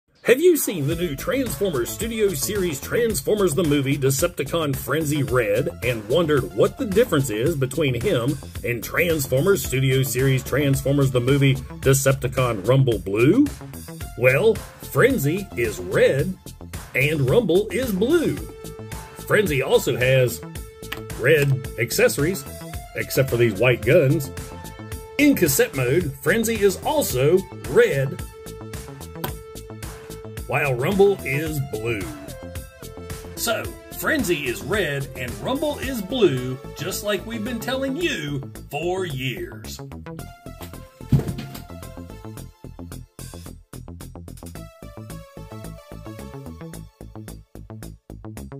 We Were Right!!!!
I just picked up a Transformers Studio Series Frenzy. and I feel vindicated.

by typing in "PATRIOT" at the top where it says "Support Your Favorite Reviewer".